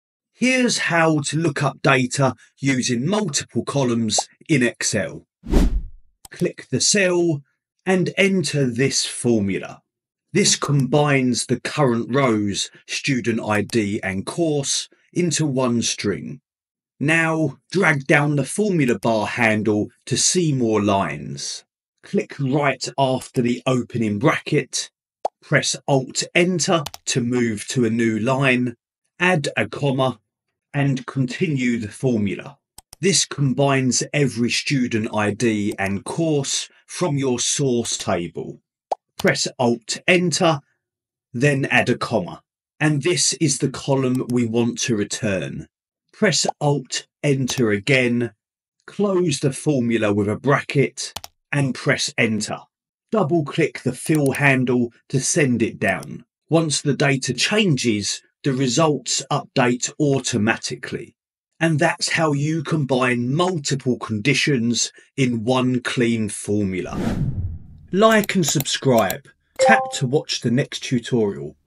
Excel Tips #96: Master Multi-Column Lookups in Excel 💡
Unlock the secrets of Excel! 🗝️ Curious about how to look up data using multiple columns? Dive into this handy guide and discover tips that will take your spreadsheet skills to the next level! 📈